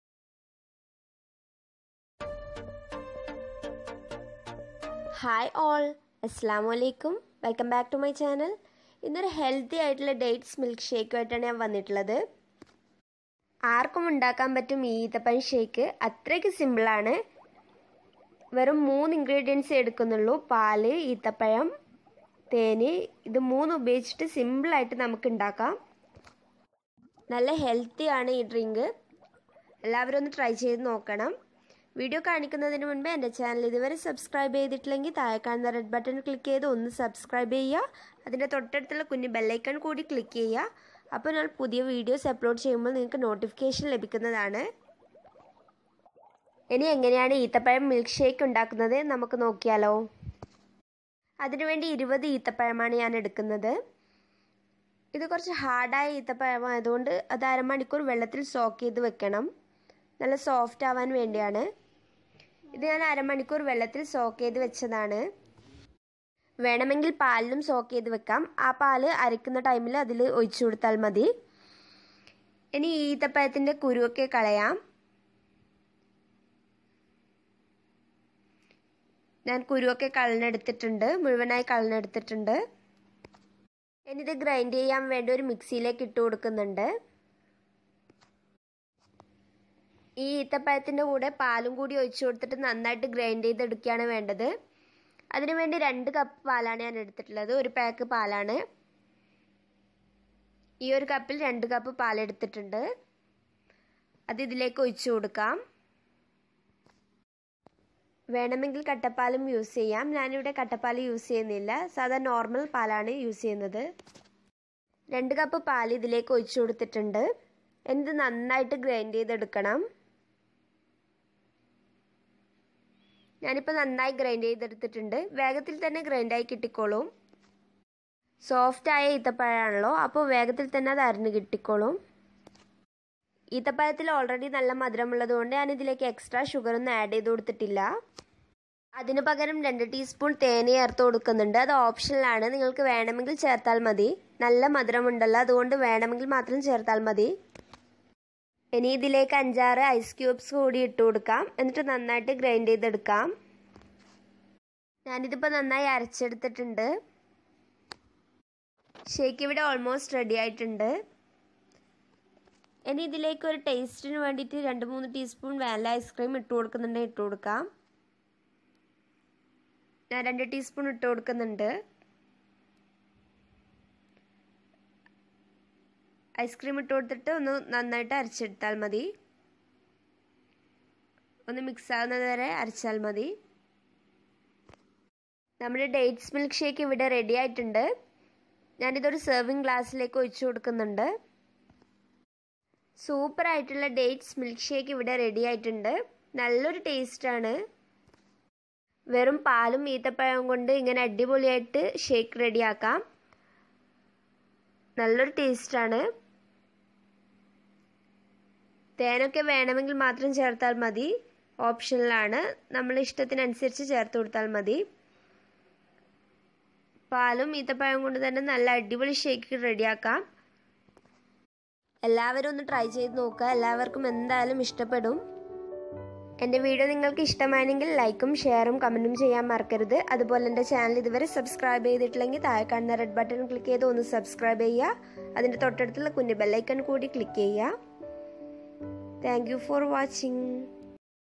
DATES MILK SHAKE//ഈന്തപ്പഴം മിൽക്ക് ഷേക്ക് ||Easy recipe in malayalam||feriz tech 'n' taste
Hey frnz..
   Today am sharing very simple nd easy dates milk shake..try it.

INGREDIENTS; ;
    Dates - 20
    Milk  -   2cup
    Honey (optional)-2tspn